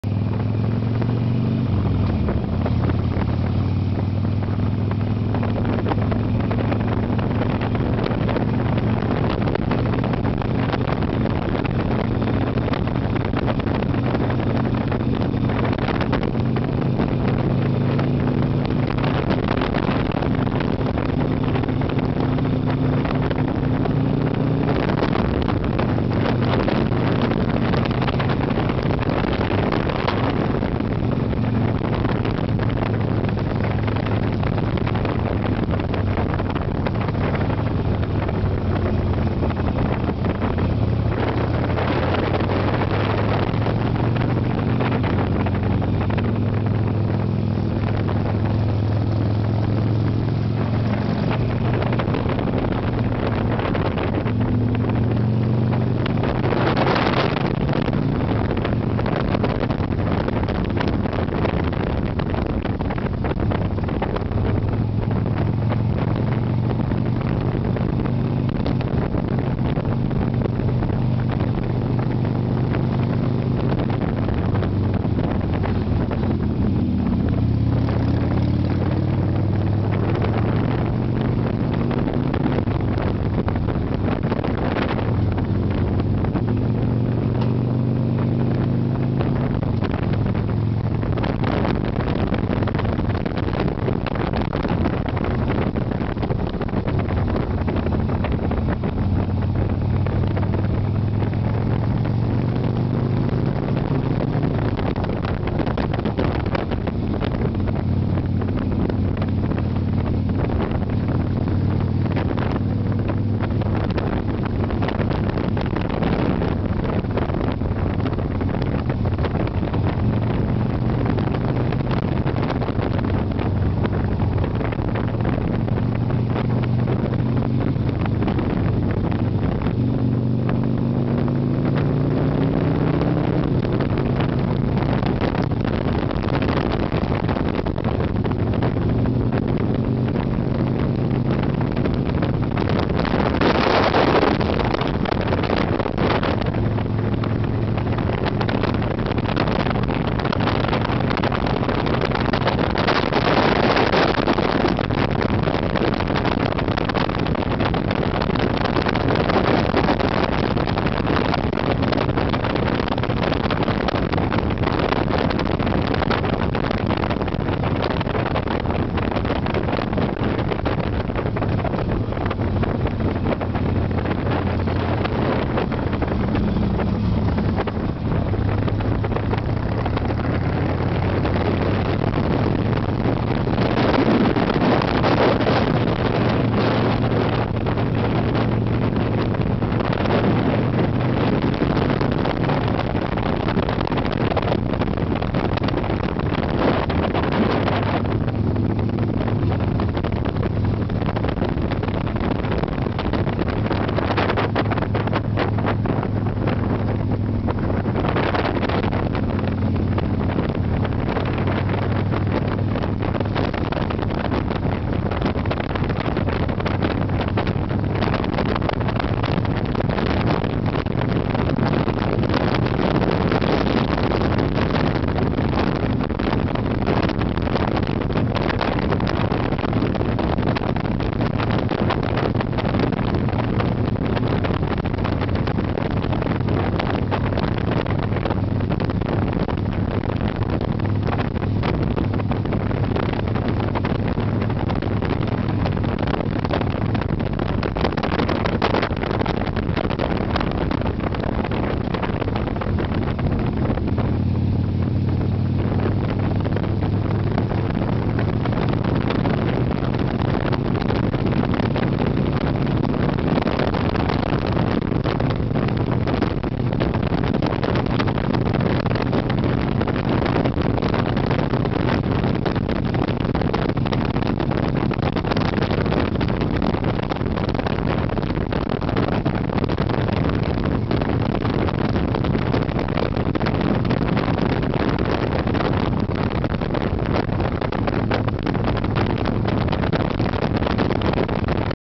estrada dos romeiros in a sportster 883
o som não está maravilhoso devido ao vento, mas dá uma idéia do passeio e das curvas gostosas desta estradinha que beira o Rio Tietê. Dá muita dó das condições do rio... é só espuma.